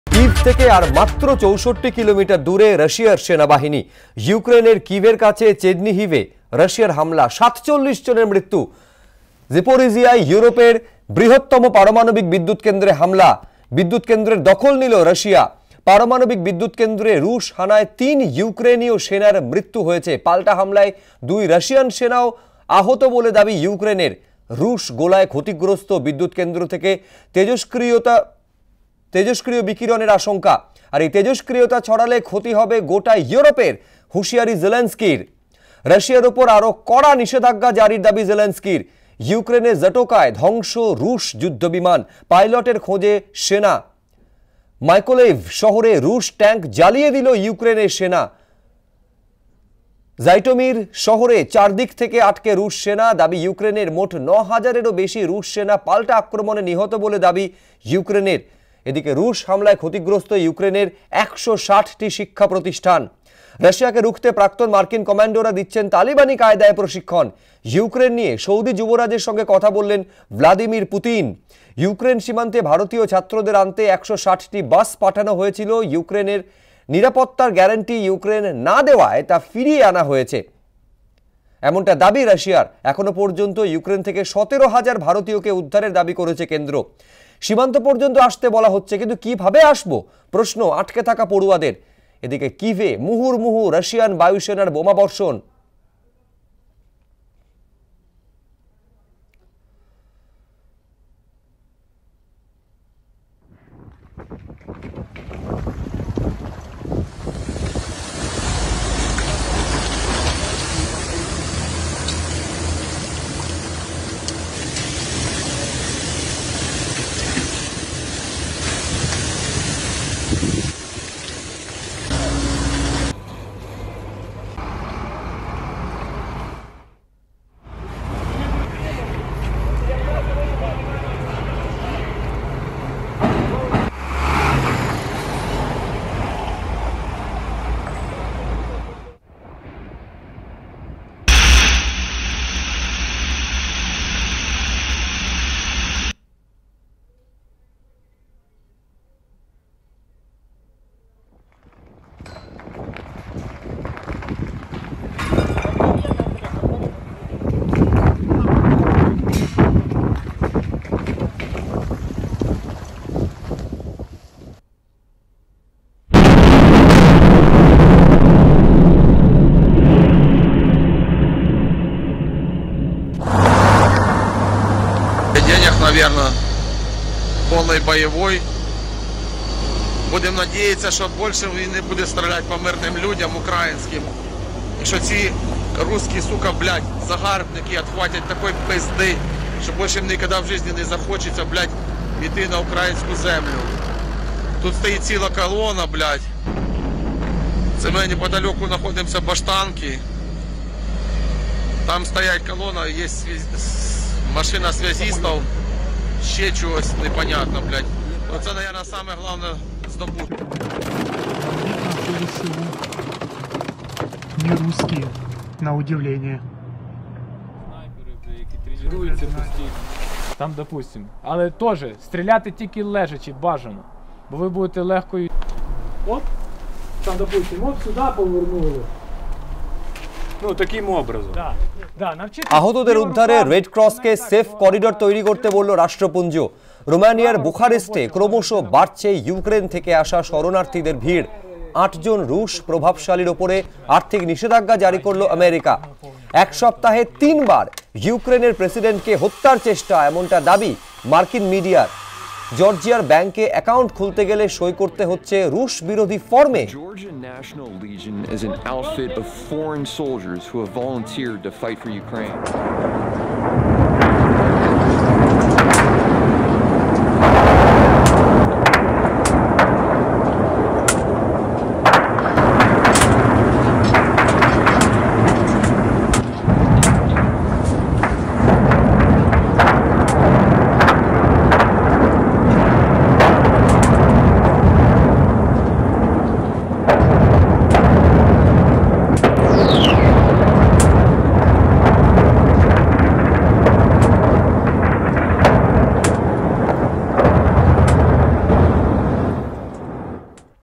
Ukraine Russia War: ইউক্রেনের কিভের কাছে চেরনিহিবে রাশিয়ার হামলা, ৪৭জনের মৃত্যু | Bangla News
কিভ থেকে আর মাত্র ৬৪ কিমি দূরে রাশিয়ার সেনা বাহিনী
ইউক্রেনের কিভের কাছে চেরনিহিবে রাশিয়ার হামলা, ৪৭জনের মৃত্যু
জেপরোজিয়ায় ইউরোপের বৃহত্তম পারমাণবিক বিদ্যুৎকেন্দ্রে হামলা
জেপরোজিয়ায় পারমানবিক বিদ্যুৎকেন্দ্রের দখল নিল রাশিয়ার